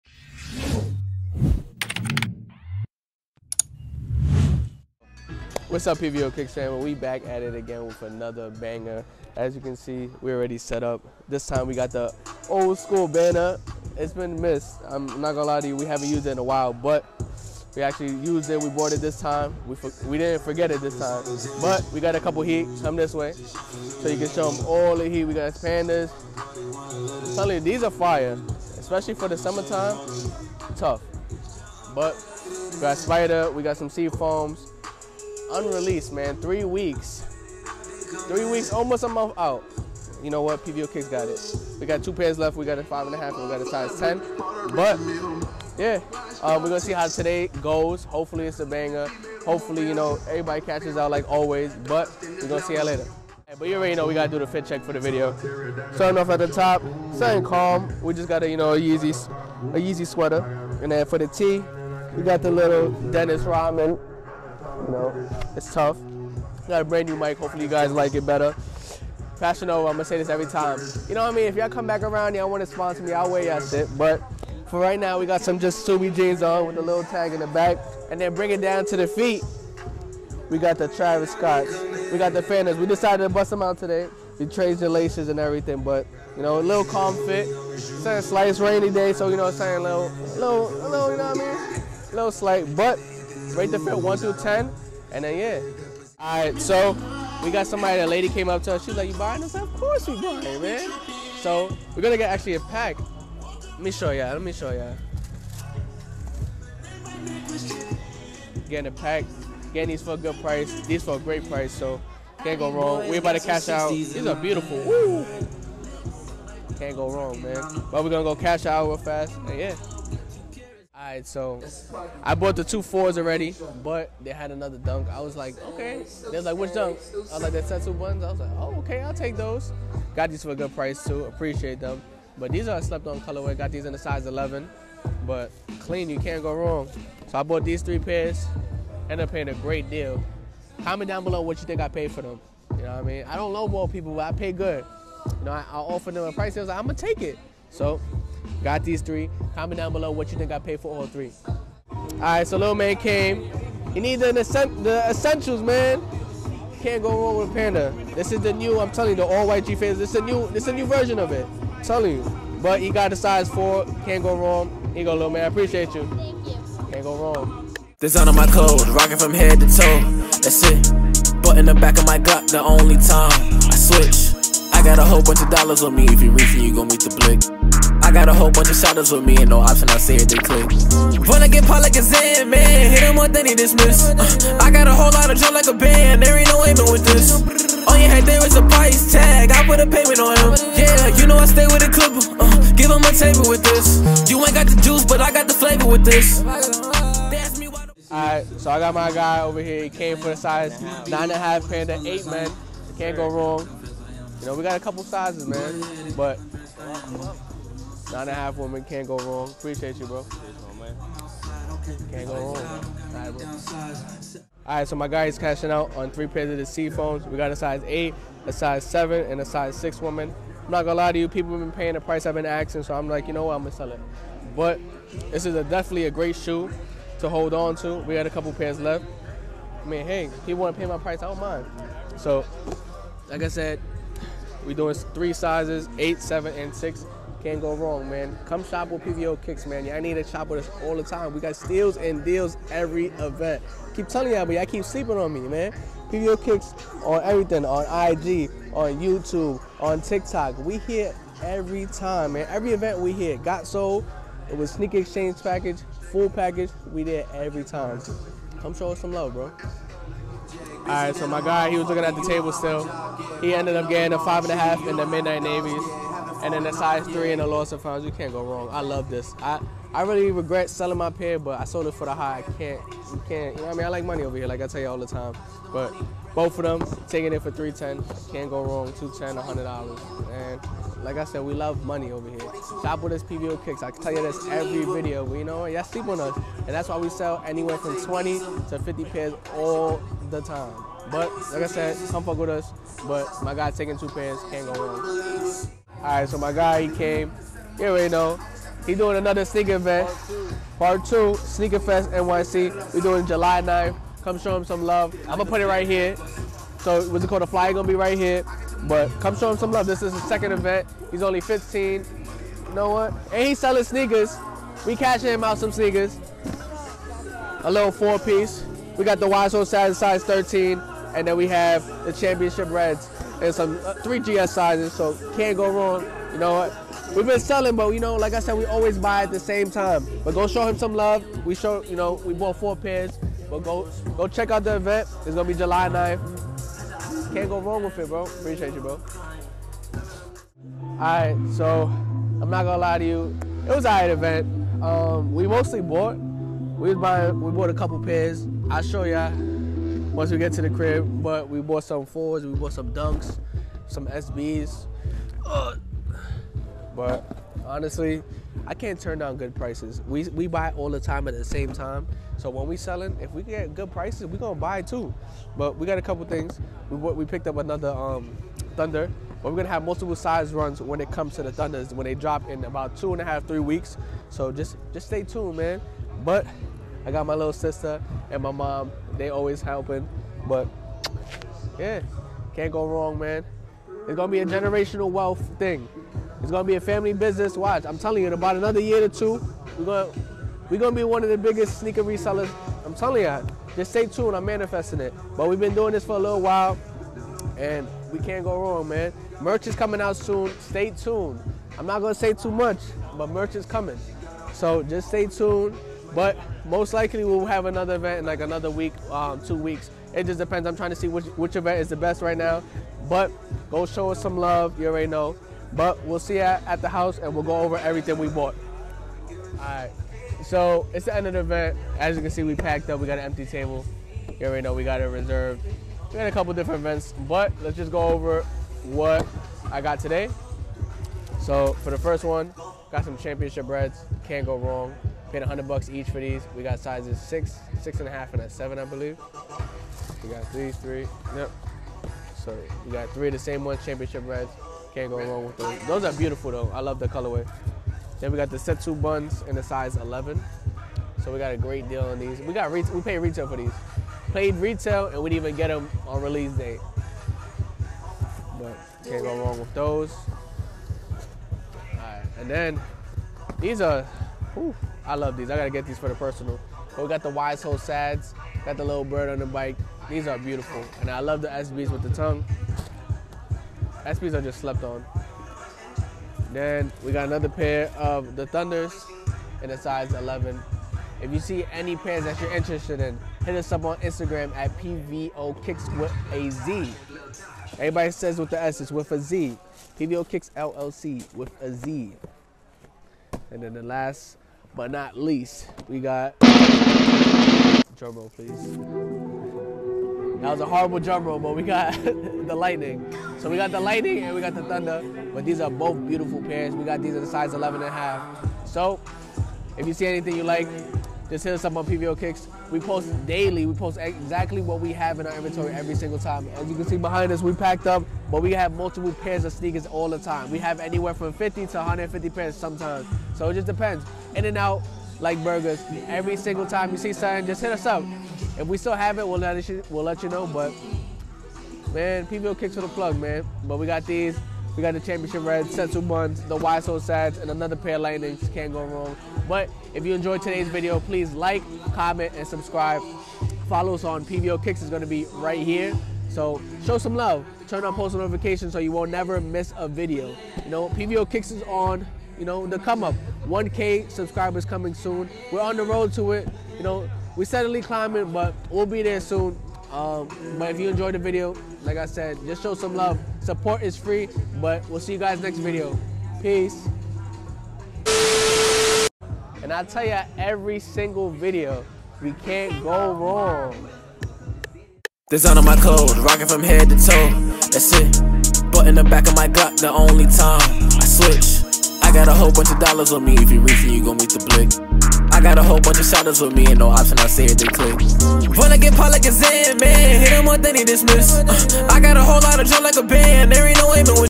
CASHING OUT AT FULLPACKAGEPROMOTIONS SNEAKER SHOW!💰
CASHING OUT at theFullPackagePromotions sneaker show! We sold multiple pairs and cashed people out! 🔥🚨

Pvokickz

Pvo Kickz